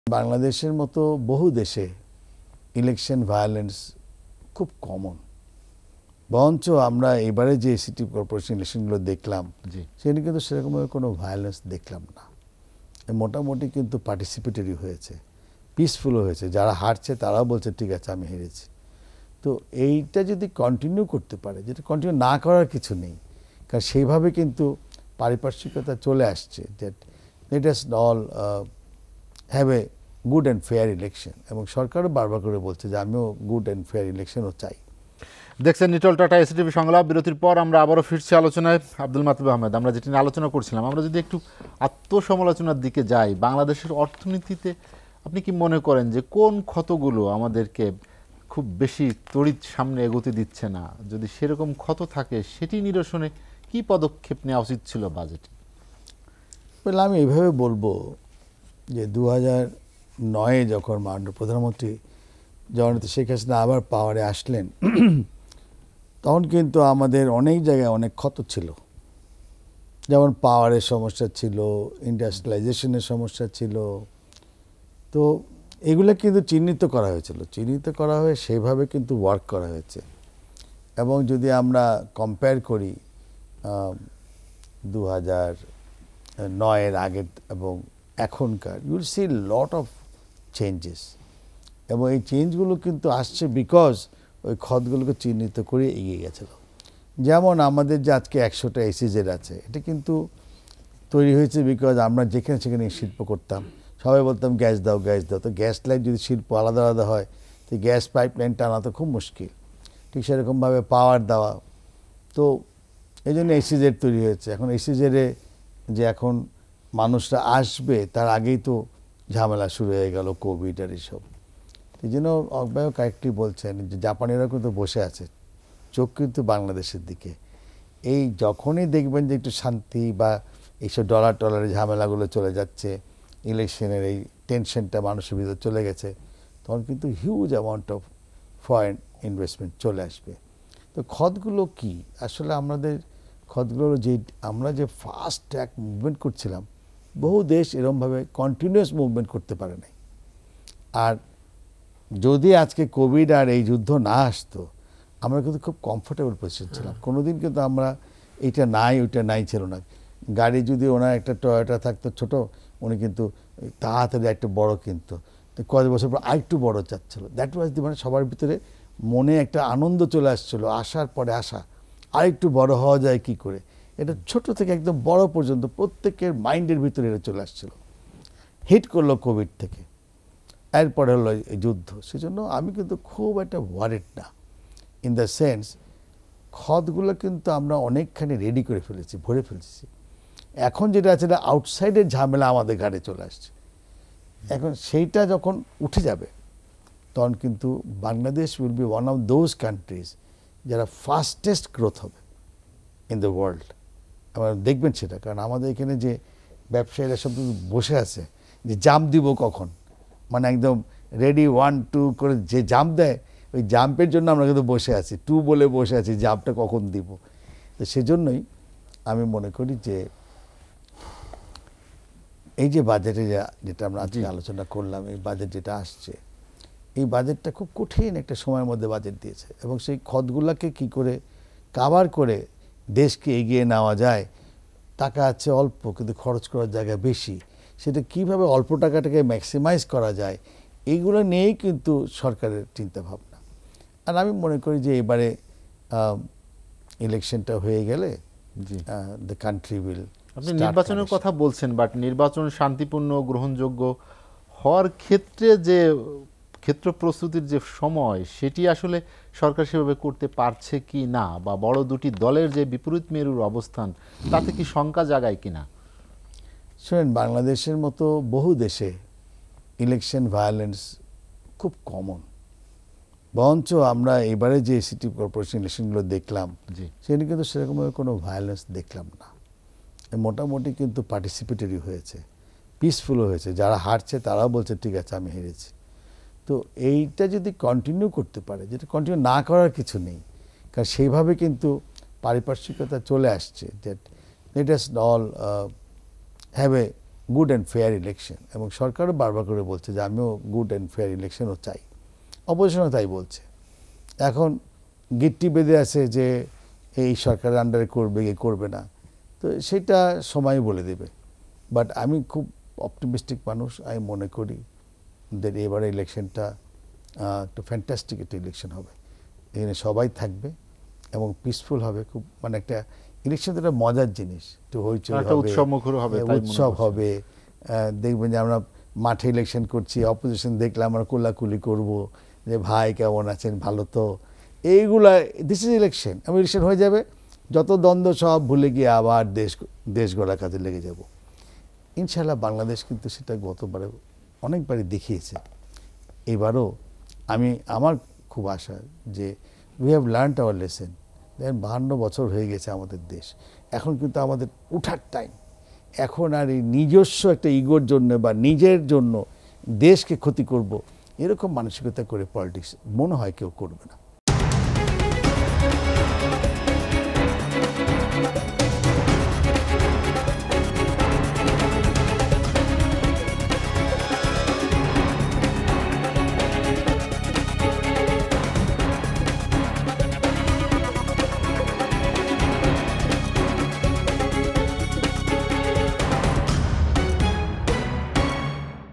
বাংলাদেশের মত বহু দেশে ইলেকশন ভায়োলেন্স খুব কমন: আবদুল মাতলুব আহমাদ | SATV
বাংলাদেশের মত বহু দেশে ইলেকশন ভায়োলেন্স খুব কমন: আবদুল মাতলুব আহমাদ

SA TV Location:
Bangladesh